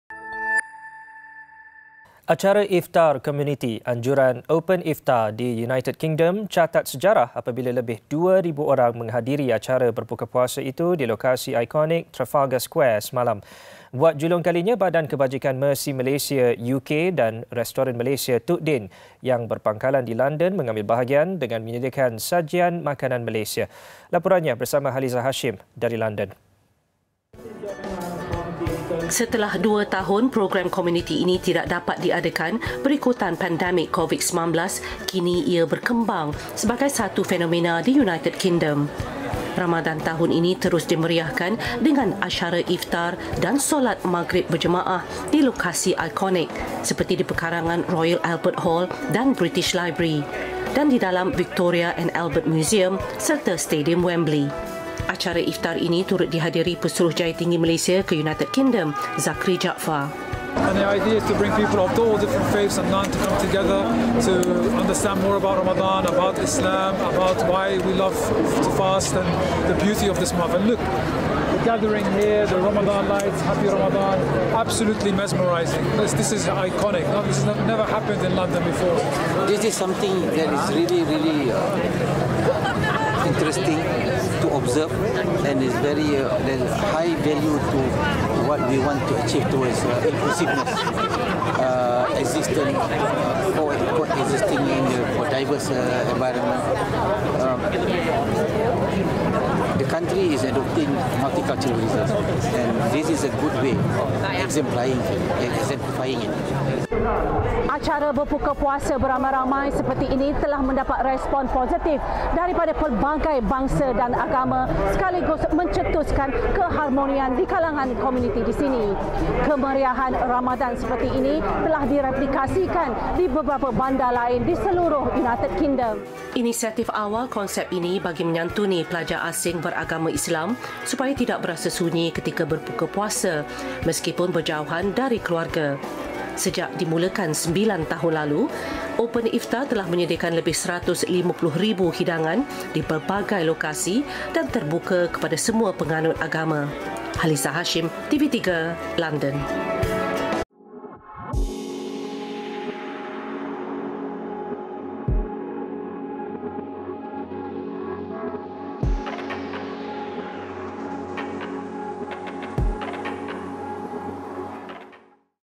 RAMADAN | Lebih 2,000 Orang Hadir Acara Iftar Komuniti
Acara iftar komuniti anjuran Open Iftar di United Kingdom catat sejarah apabila lebih 2,000 orang menghadiri acara berbuka puasa itu di lokasi ikonik Trafalgar Square, semalam.

Buat julung kalinya Badan Kebajikan Mercy Malaysia UK dan restoran Malaysia, TukDin yang berpangkalan di London mengambil bahagian dengan menyediakan sajian makanan Malaysia.

Laporannya bersama Haliza Hashim dari London.